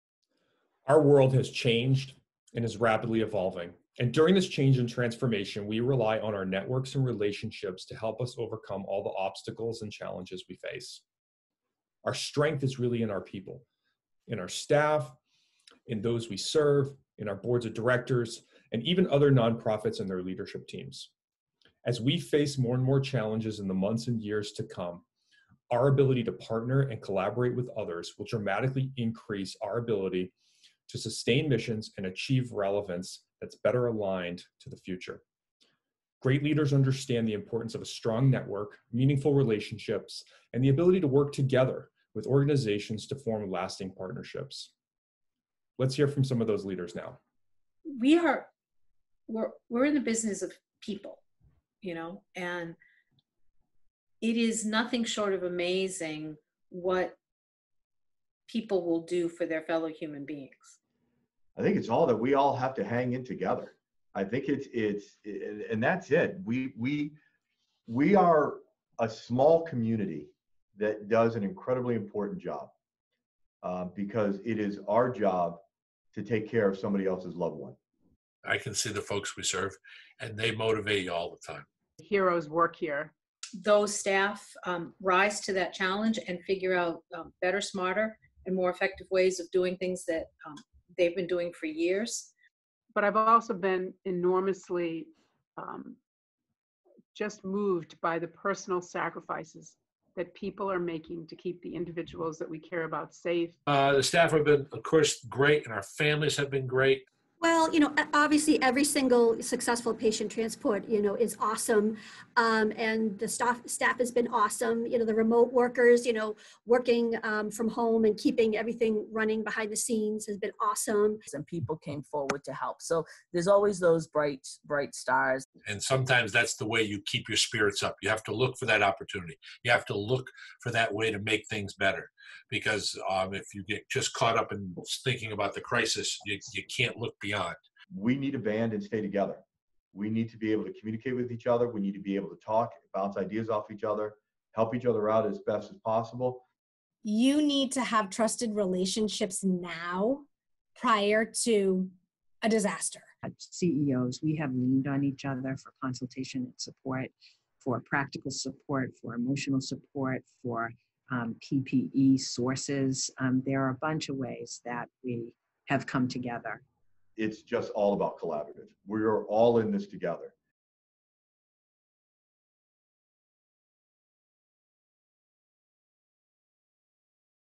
Networks, Relationships, & Relevance
A HUGE Thank You to all the CEOs that took part in discussions over the past several months!
This video provides CEO Insights on the importance of relationships and partnerships.

LINKS -